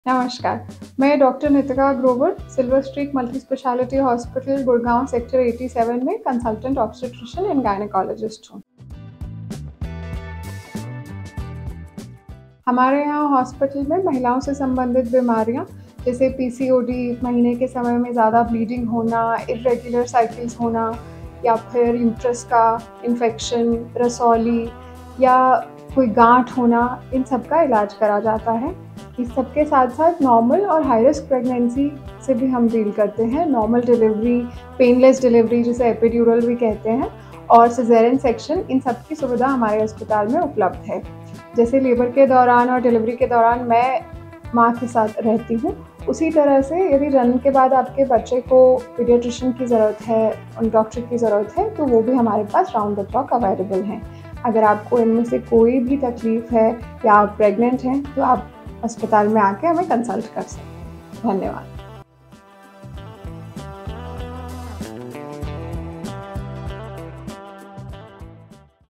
महिलाओ से सम्बंदित सभी बीमारियों का ईलाज Silver Streak Hospital में 24 घंटे उपलब्ध है
Dr. Nitika Grover
Consultant Obstetrician and Gynaecologist, Silver Streak Multispeciality Hospital Gurugram

✅ Our Address - Silver Streak Multispeciality Hospital, Opp. DLF Sky Court, Sector 87, Gurugram